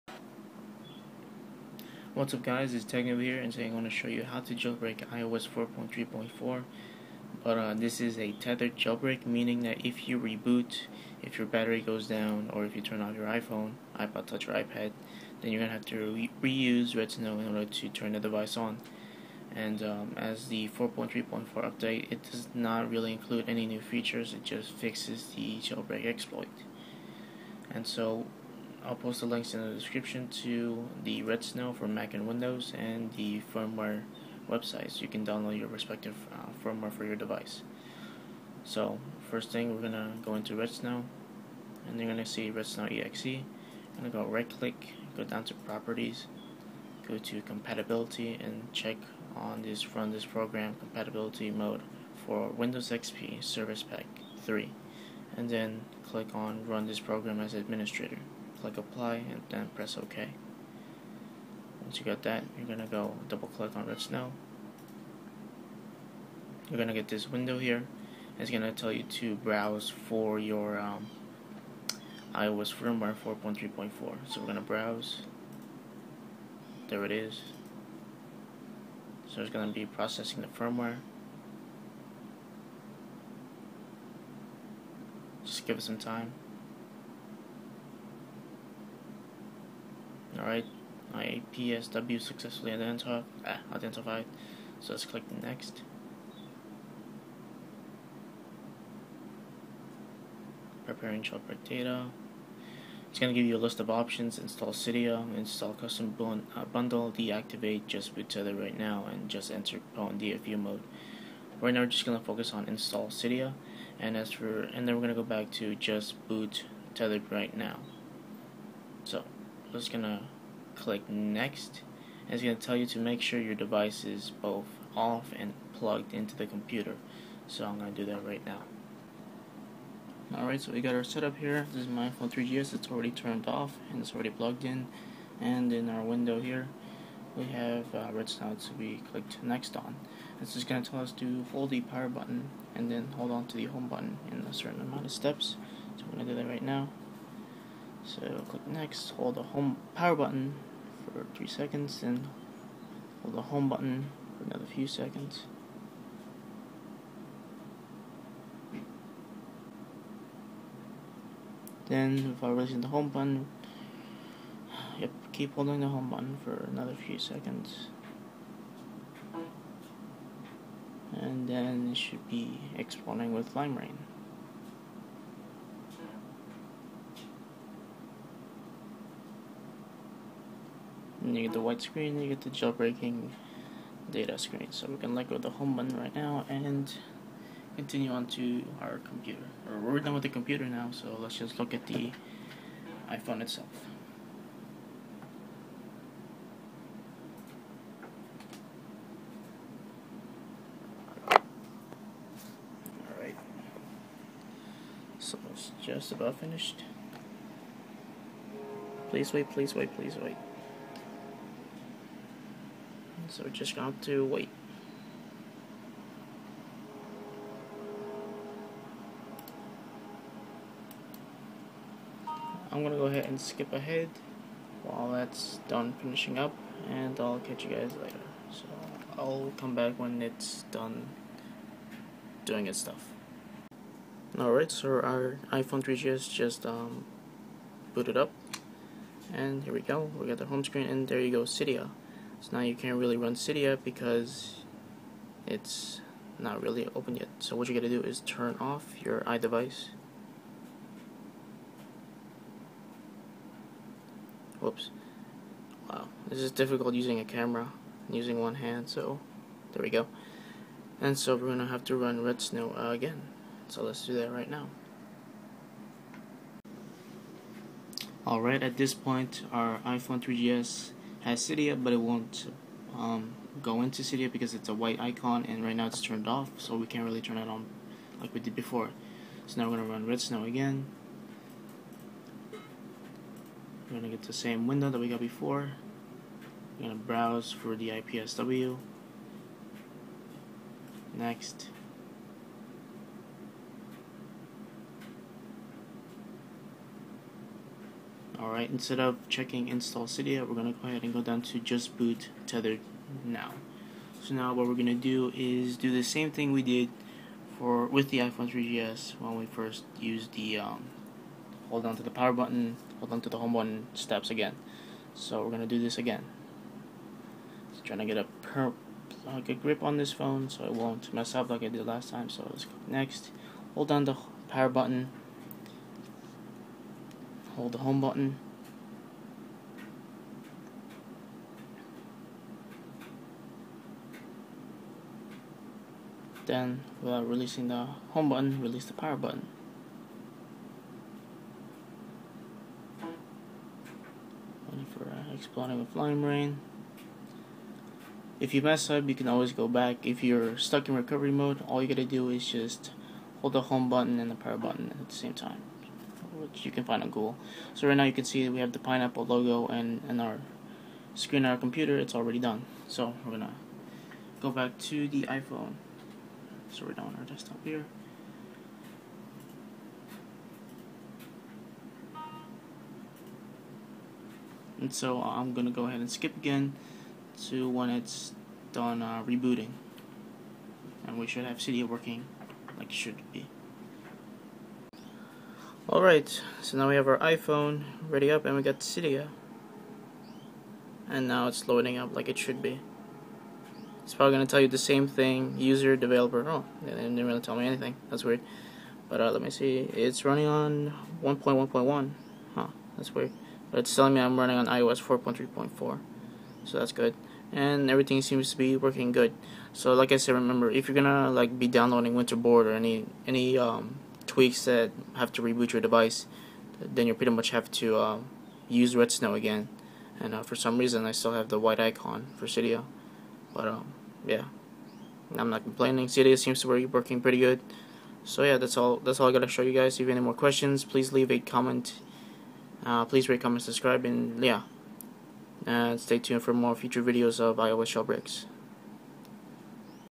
iOS 4.3.4/4.3.5 Tethered Jailbreak for iPhone, iPod touch, and iPad
Whats up guys, today Muscle Nerd releases a redsn0w beta that jailbreaks iOS 4.3.4
This jailbreak is only tethered at the moment.
So far this only works on the
    iPad 1
    iPhone3GS
    iPhone4 (GSM)
    iPhone4 (CDMA)

Links
Redsn0w (Windows + Mac)

Everytime you have to reboot or turn on the device, you will need to use Redsn0w like I showed in the video